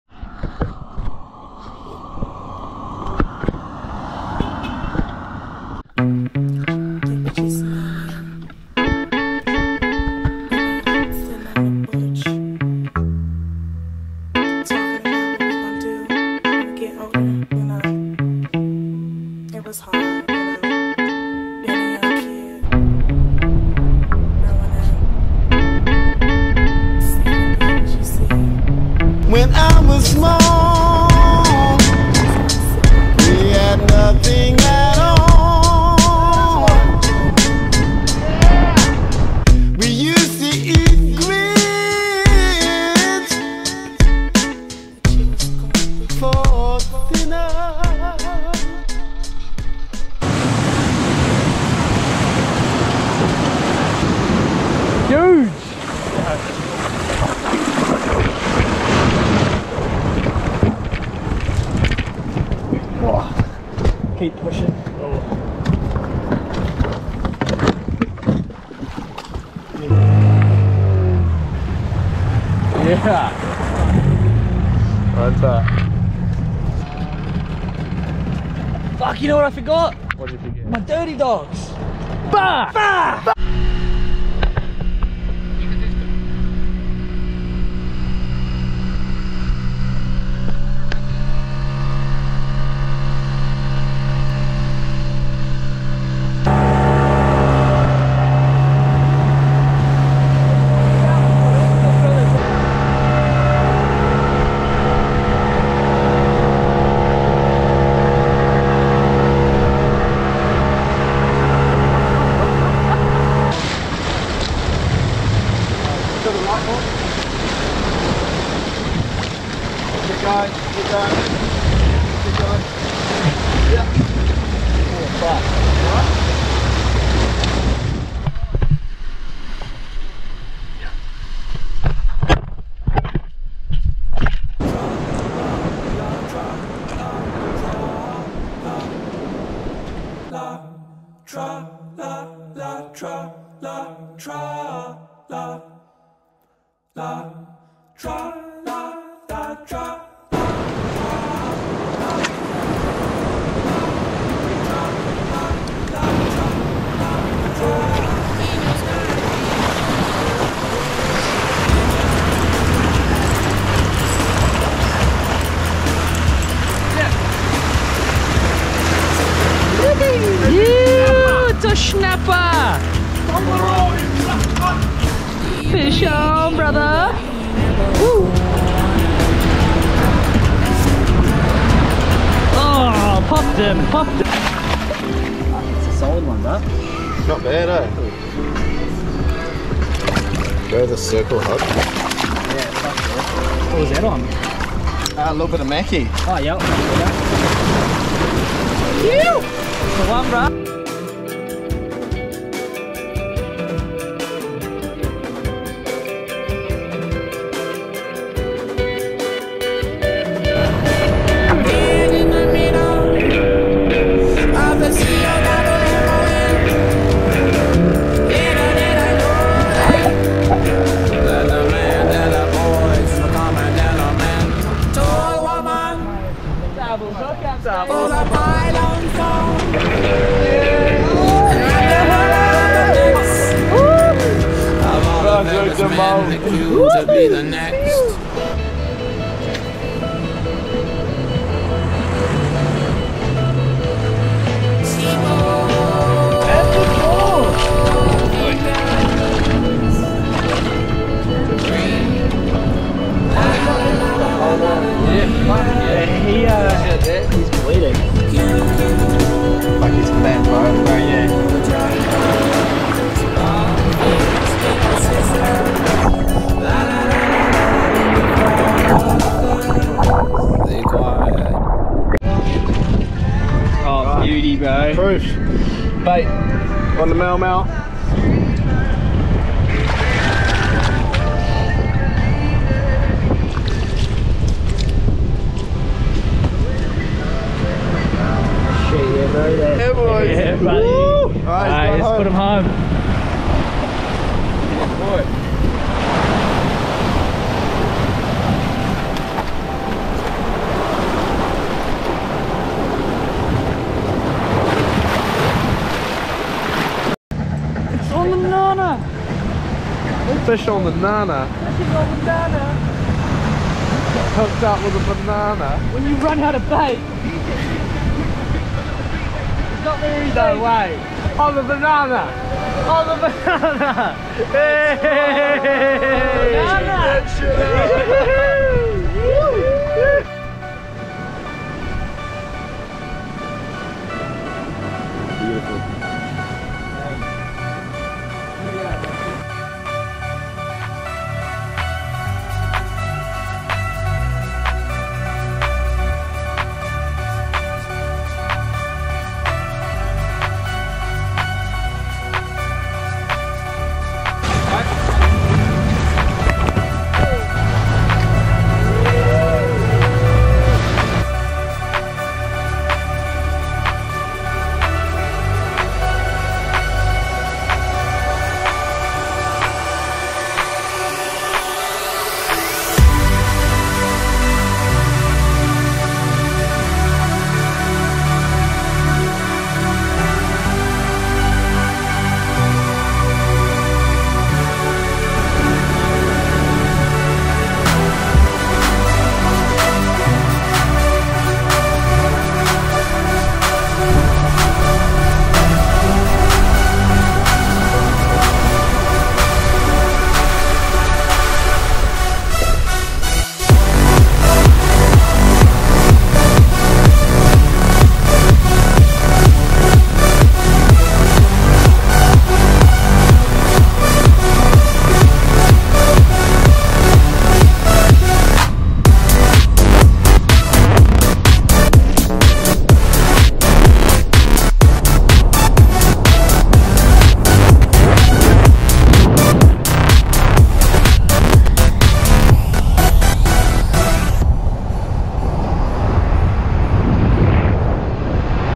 Rock Fishing - Coromandel Snapper caught on Banana
The morning starts from Whitianga with an early wake up and a quick stop at the Coromandel bakery. Supplies all packed and off we go to Fletchers Bay. With a short motor around the corner to The Panicles we get right into fishing. Plenty of snapper to go around but the craziest thing happens when I chuck a banana peel on the hook for a bit of a laugh...